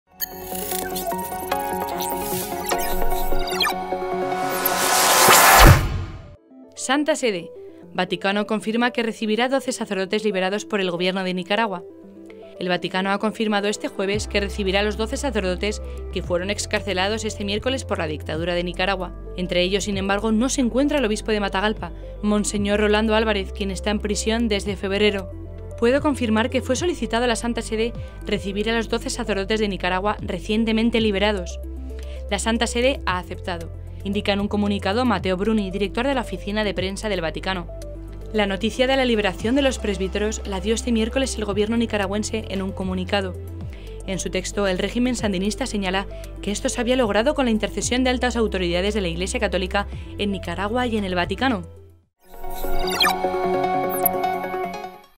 Vídeo Noticia: Vaticano confirma que recibirá a 12 sacerdotes liberados por el gobierno de Nicaragua
El Vaticano ha confirmado este jueves que recibirá a los doce sacerdotes que fueron excarcelados este miércoles por la dictadura de Nicaragua. Entre ellos, sin embargo, no se encuentra el Obispo de Matagalpa, Mons. Rolando Álvarez, quien está en prisión desde febrero.